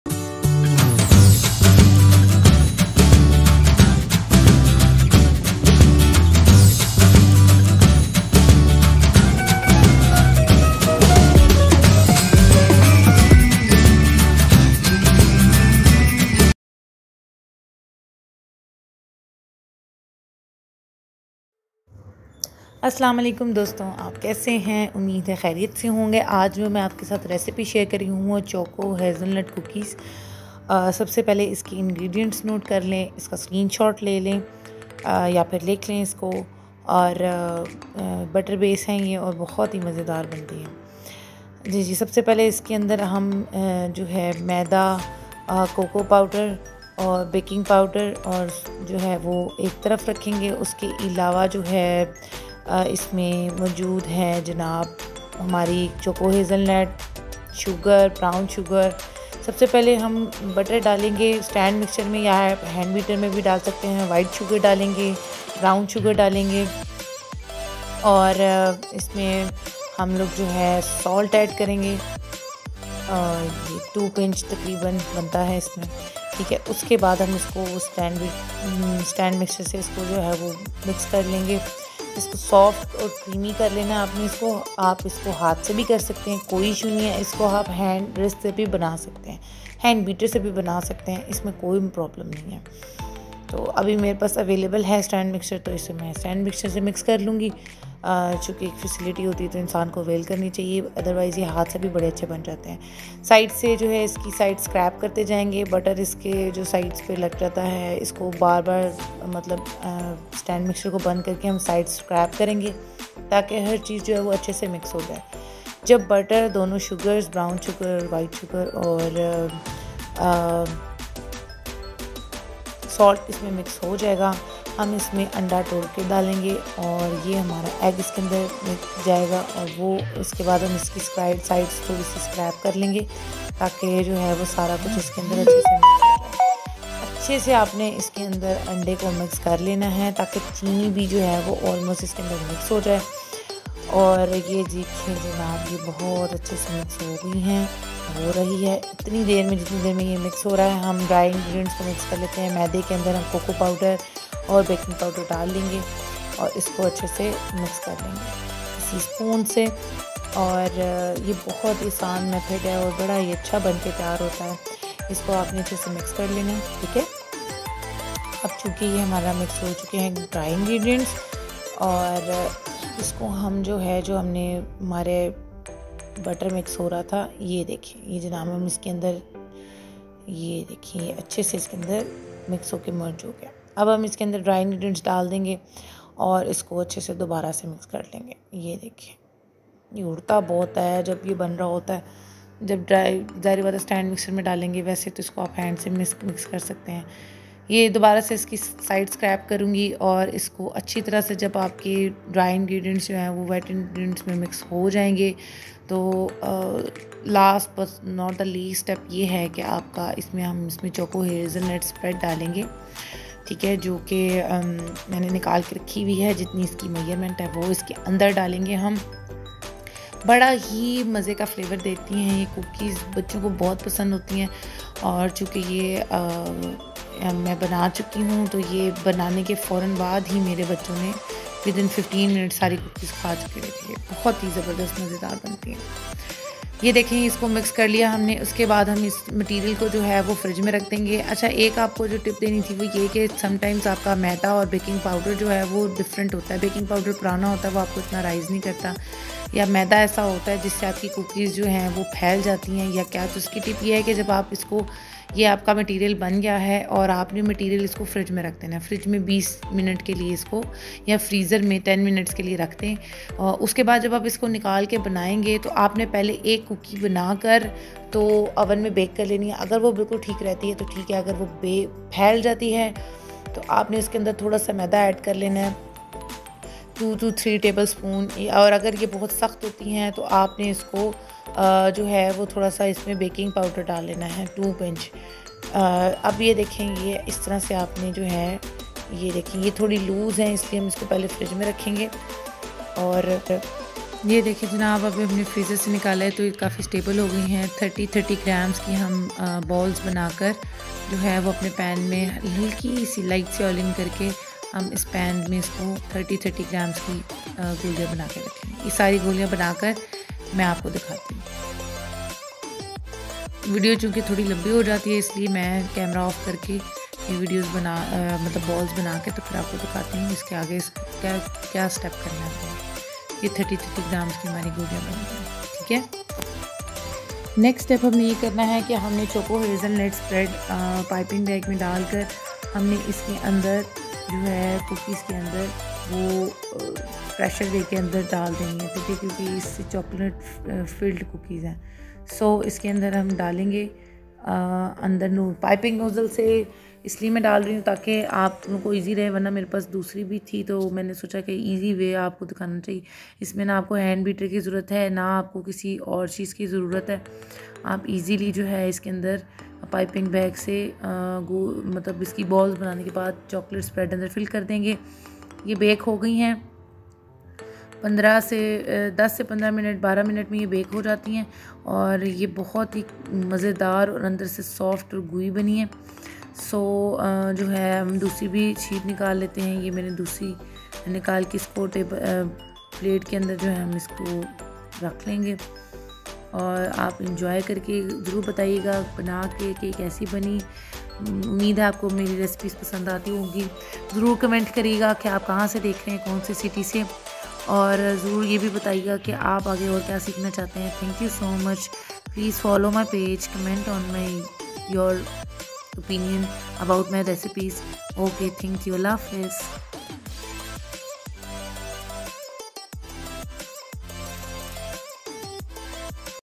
chocolate hazzelnut filled cookies by sweettreatsbywardah
Chocolate hazelnut filled cookies:
Butter : 100g
caster sugar : 50g
brown sugar :75 g
salt :1 pinch
vanilla :1 tsp
Flour : 150g
coco powder :5g
Chocolate hazelnut :50 g
baking soda :2g
egg : 1
Choco hazelnut spread :  for filling